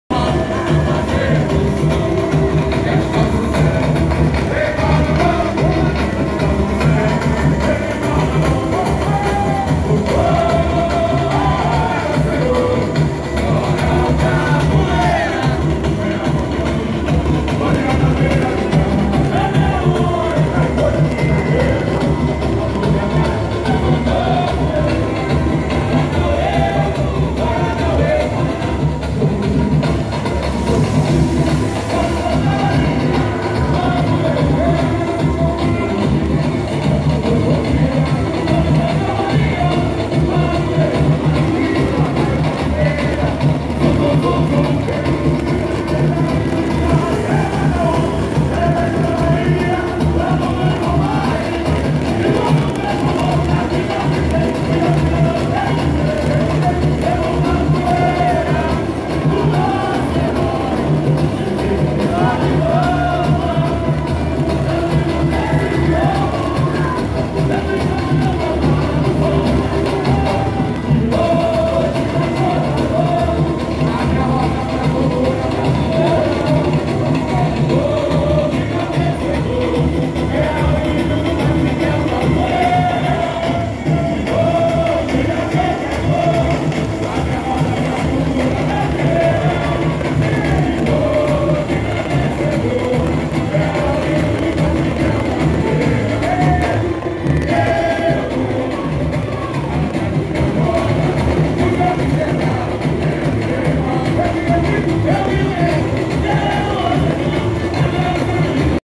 UPM - UNIDOS DE PADRE MIGUEL 2020 - TRIPÉ "GINGA"
TRIPÉ "GINGA"

Ginga é a valorização do povo afro-brasileiro, a ala representa o culto aos bois referindo-se ao cotidiano da tribo Mocupe com os guerreiros angolanos e os bois africanos numa alusão à coroação do Boi Vermelho UPM.